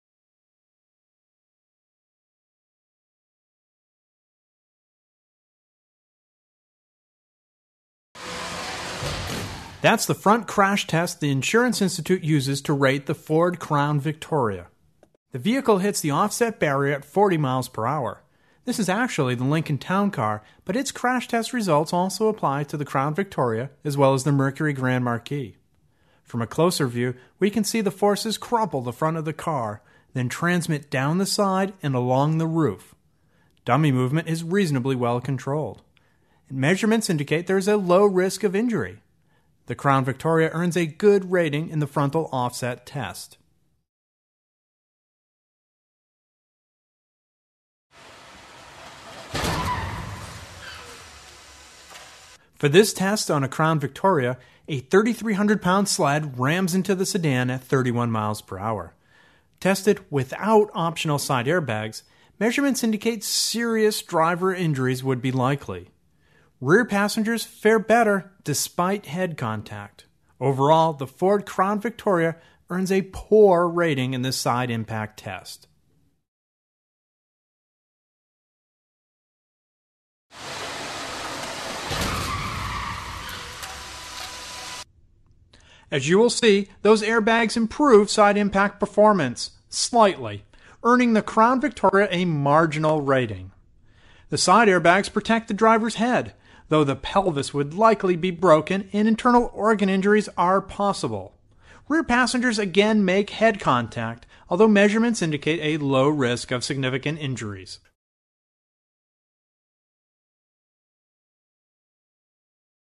2003-2010 Ford Crown Victoria - IIHS Crash Tests
The Insurance Institute for Highway Safety's Front and Side Impact crash tests for the Ford Crown Victoria, as presented by Consumer Reports

The Ford Crown Victoria received the following ratings in the IIHS's crash tests:

Frontal Offset: Good
Side Impact (No Airbags): Poor
Side Impact (With Airbags): Marginal